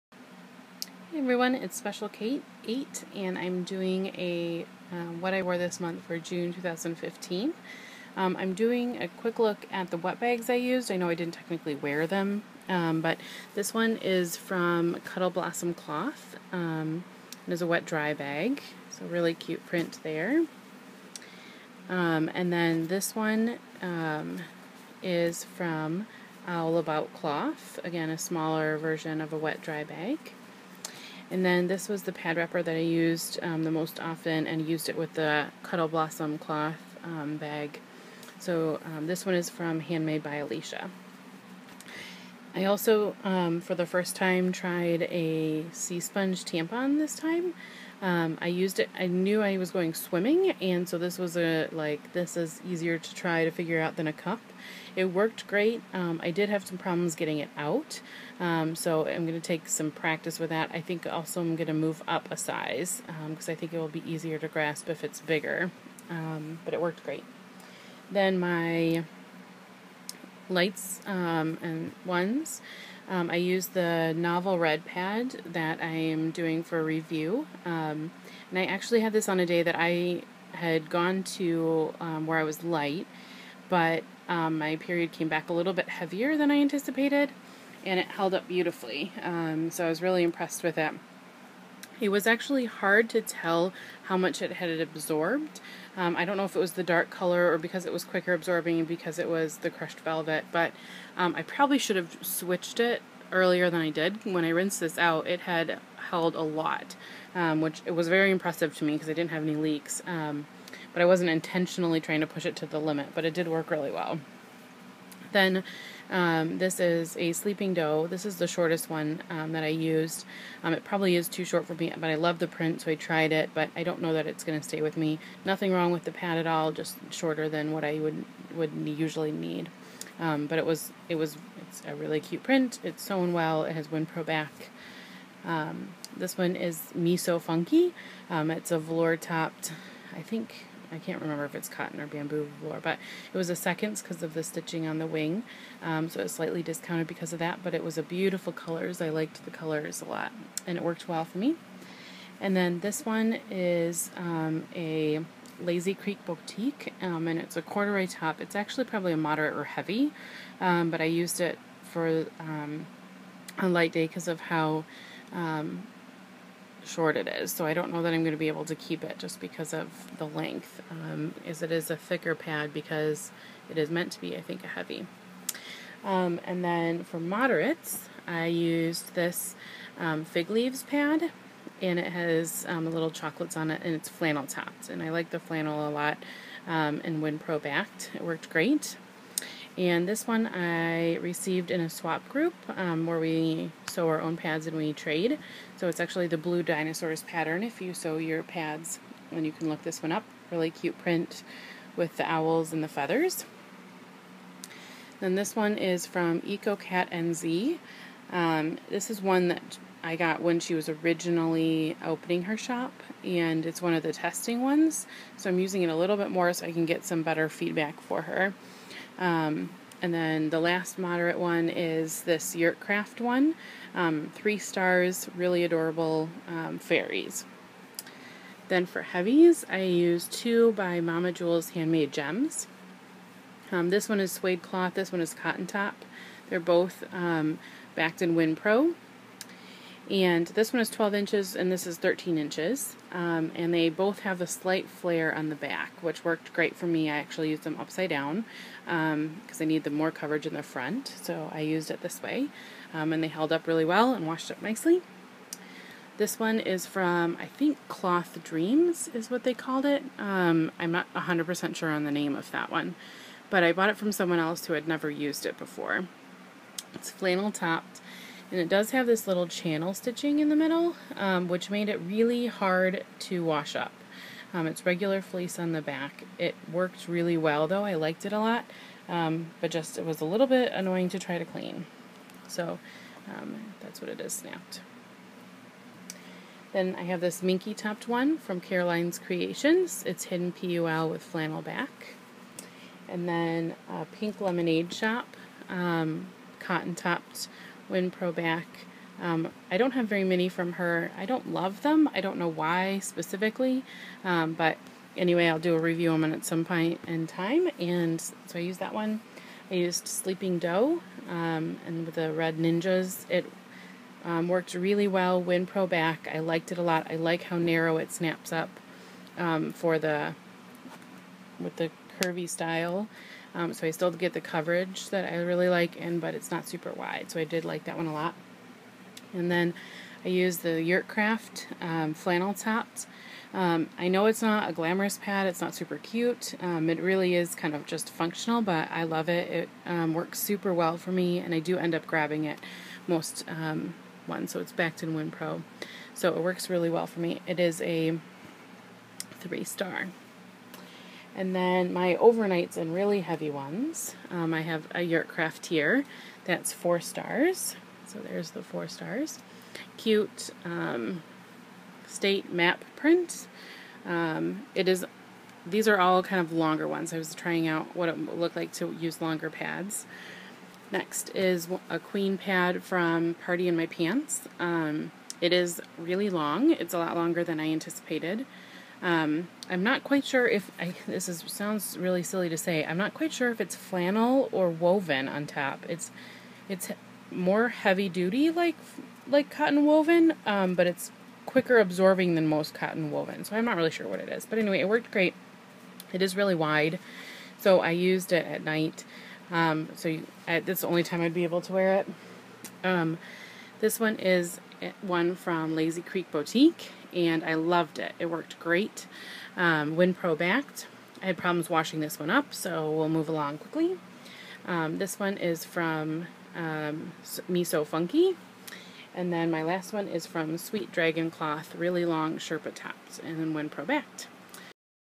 WIWTM June 2015
Cloth Pads and other related items I used for my cycle for June 2015.

Featured wetbags:
Cuddle Blossom Cloth
Handmade by Alycia

Featured pads:
Novel Red
Sleeping Doe
Cloth Dreams (yes, it is Cloth Dreams, I verified it!)
The Silver Liner (not Sweet Dragon Cloth, sorry) not open right now